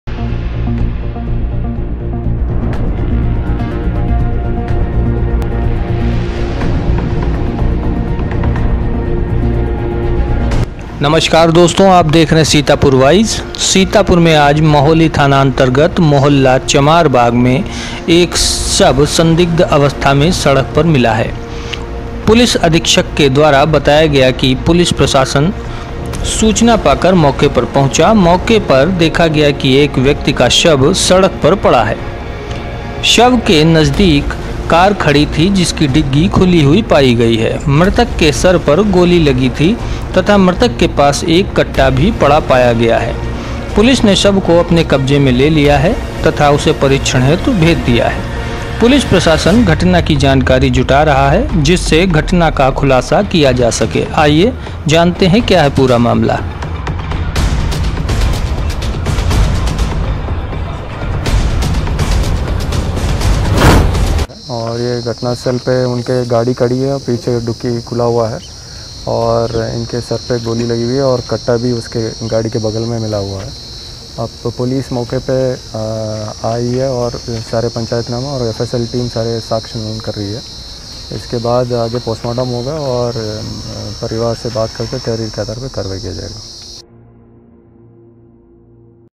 SITAPUR NEWS :-सीतापुर के महोली क्षेत्र में सड़क पर पड़ा मिला युवक का शव।
सीतापुर के महोली क्षेत्र में सड़क पर पड़ा मिला युवक का शव। पुलिस प्रशासन घटना की कर रहॉ है जॉच।

Report : Er. Rajesh Shukla
Sitapur Voice NEWS